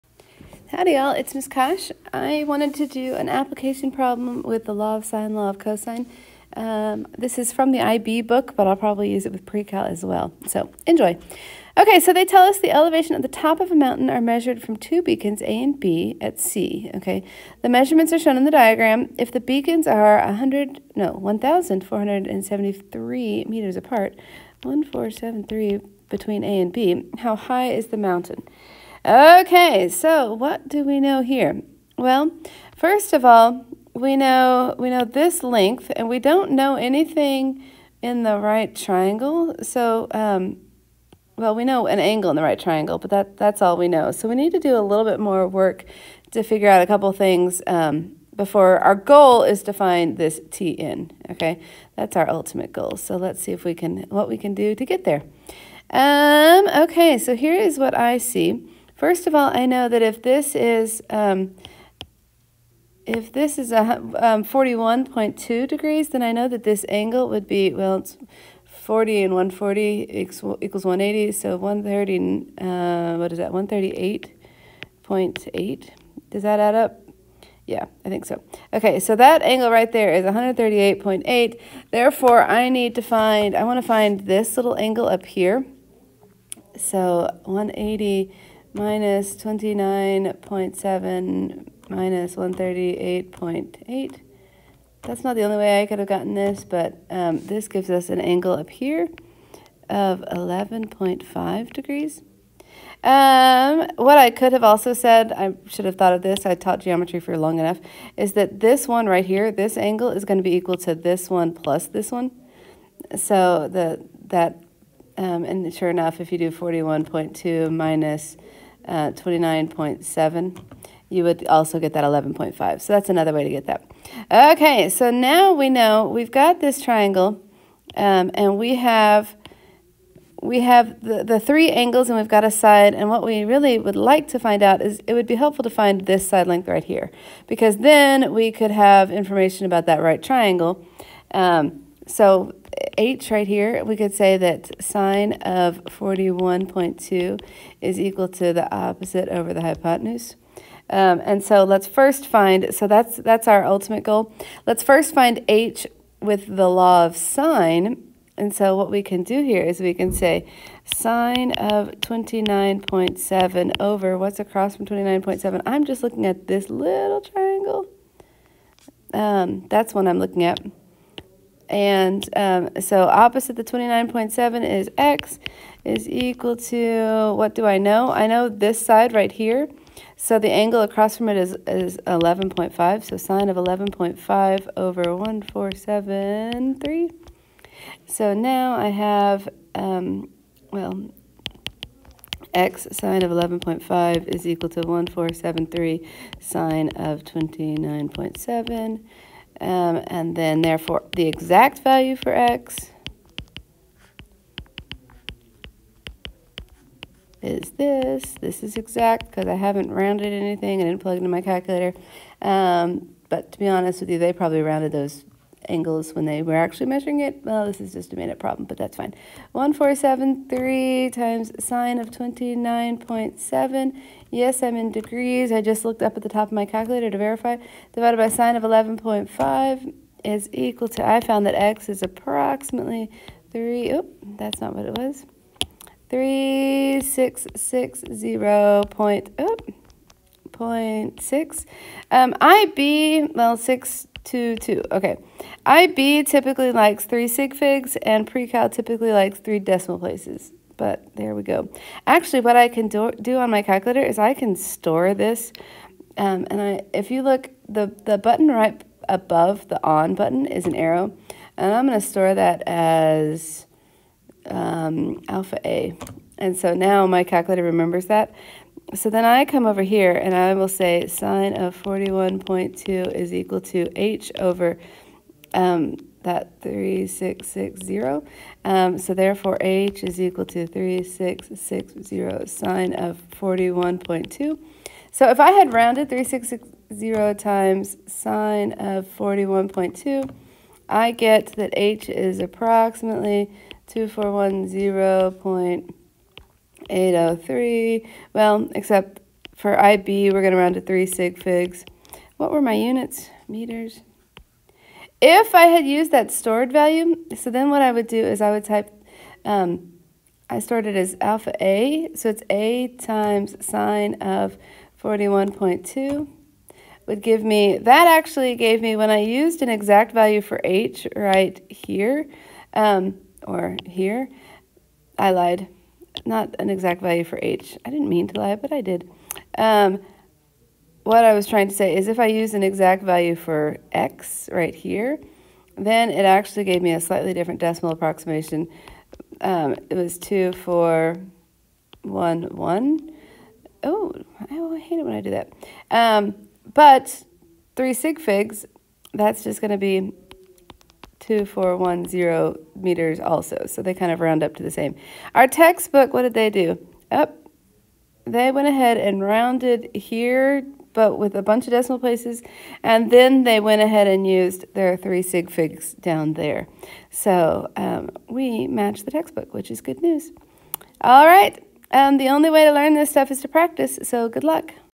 Law of Sine Application Problem
Ms. Kosh works an application problem for IB Math and PreCal.